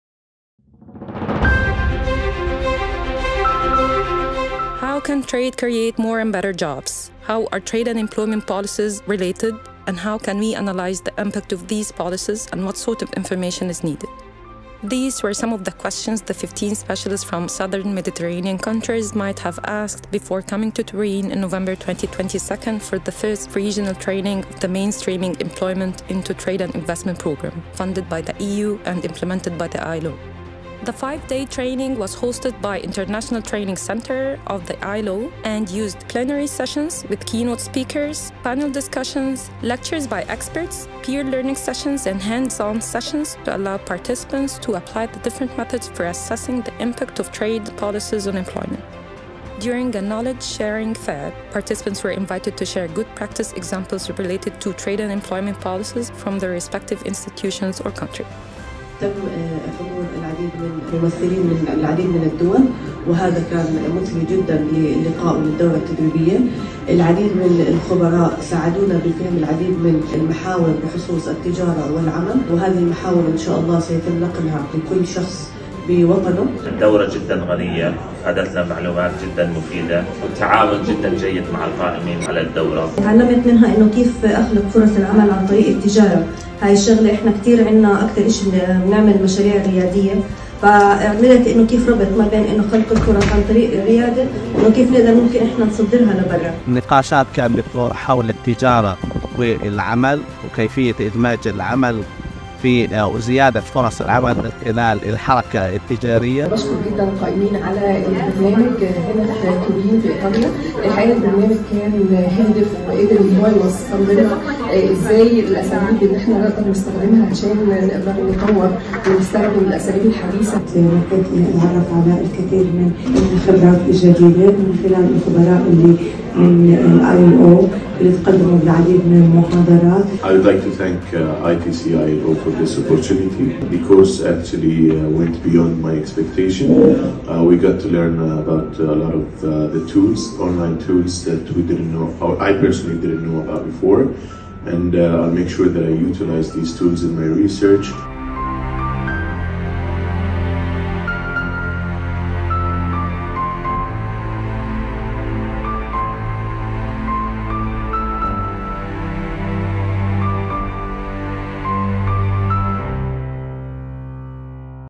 Regional Technical Training on Trade and Employment
The Mainstreaming Employment into Trade and Investment (METI)  project conducted its first regional technical training on trade and employment.
The 5-day training brought together technical experts from the Southern Mediterranean countries and was held from 31 October to 4 November 2022 at the International Training Centre of the ILO (ITCILO) in Turin.